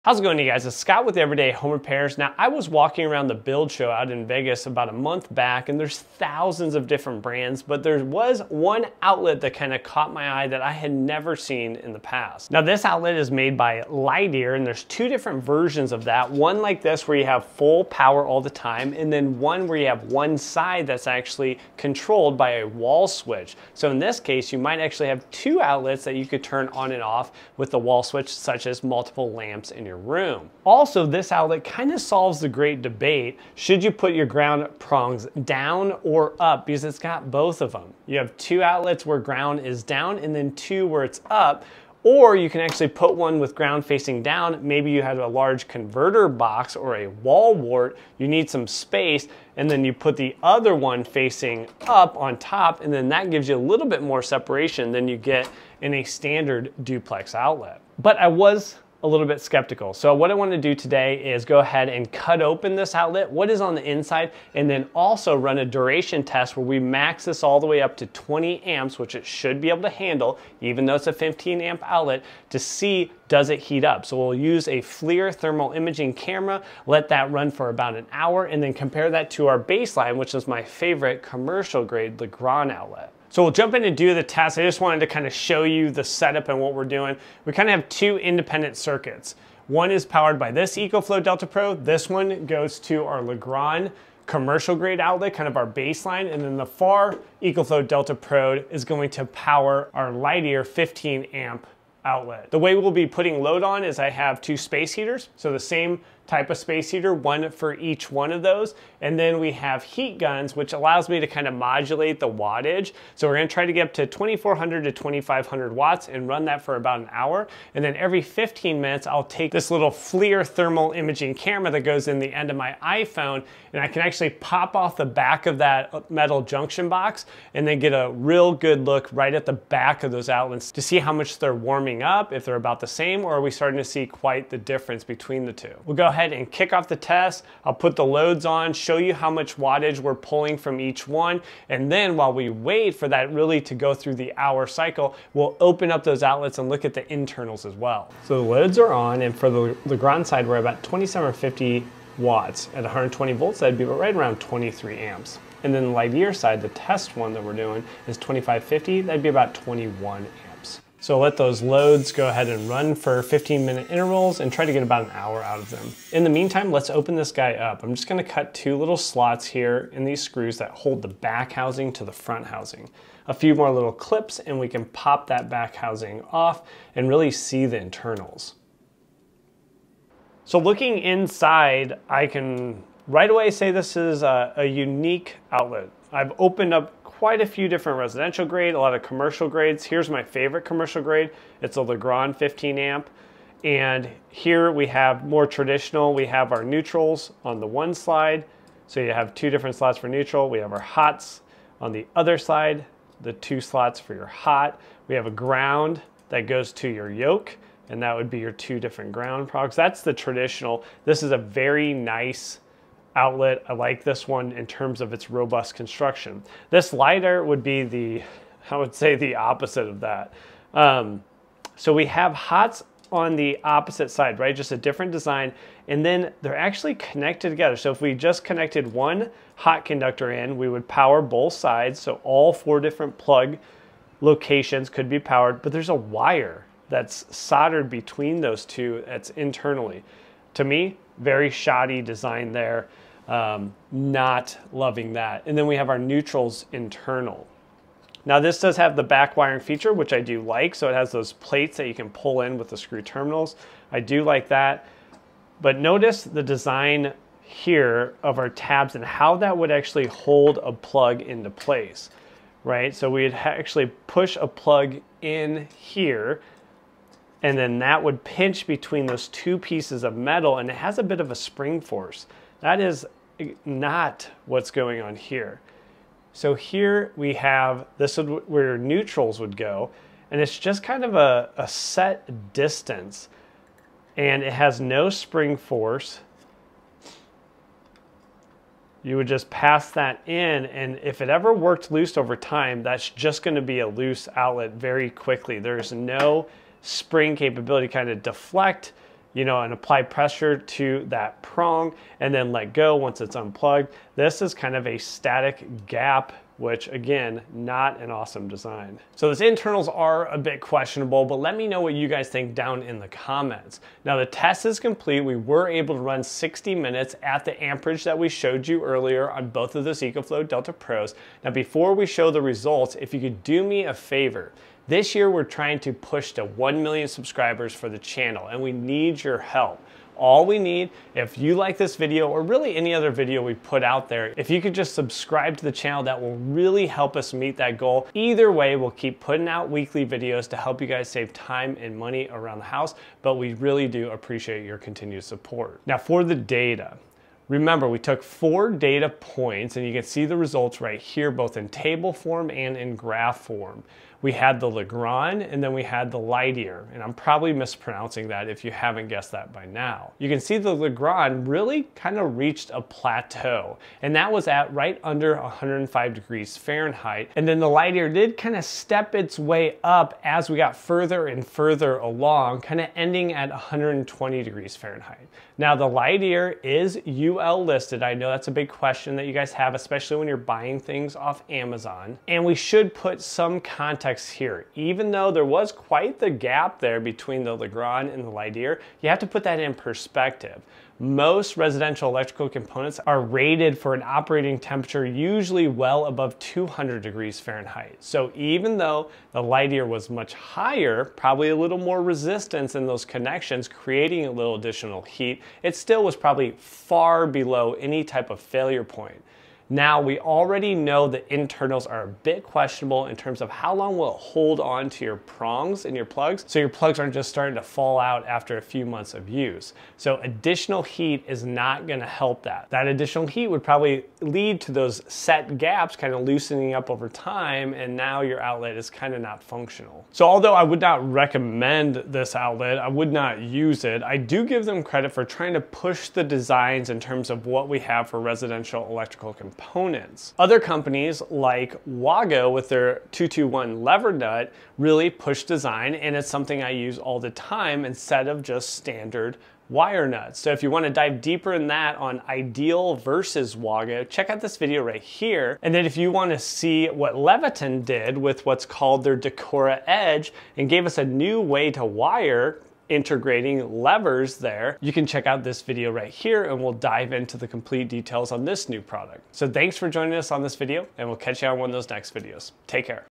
The Most Innovative Outlet You've Never Seen | Full Load Testing
LIDER Outlet
LIDER Switchable Outlet
All My Favorite DIY Electrical Tools

I found this new outlet design at the International Builder Show (IBS) 2024 in Las Vegas.  I decided to buy several from Amazon and give them a bit of a test.  I was surprised with what I found on the inside and also did a bit of a max load test for 60 minutes to see what the internal temperatures would reach and if this new outlet design is safe for your home.